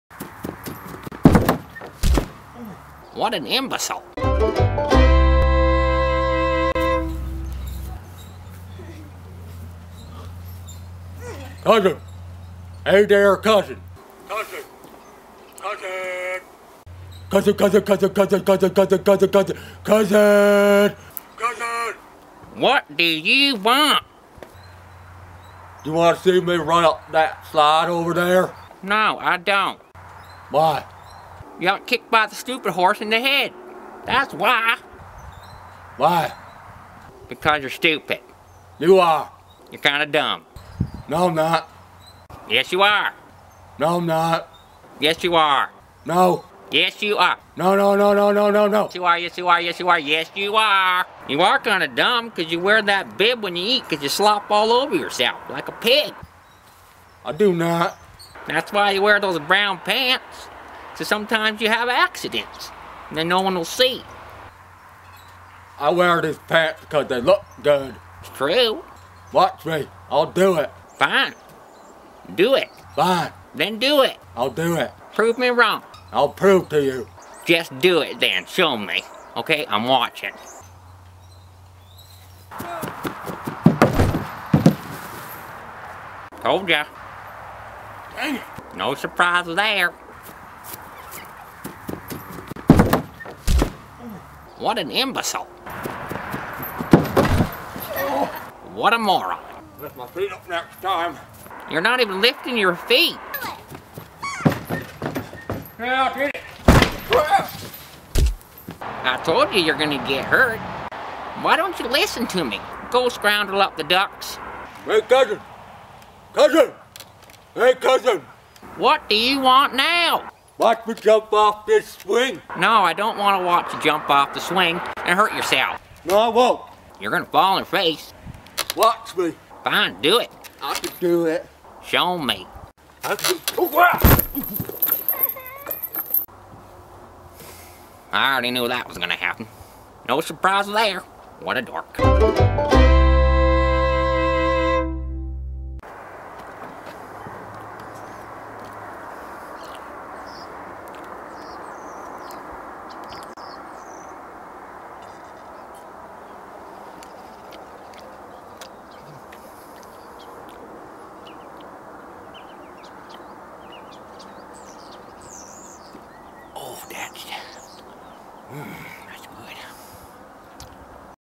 RUN UP THAT SLIDE
Howdy Y'all. Watch My cousin Ed RUN UP THAT SLIDE and fail!   These FUNNY SKITS are perfect for the REDNECK in you! Hope you in joy me COMEDY SKITS.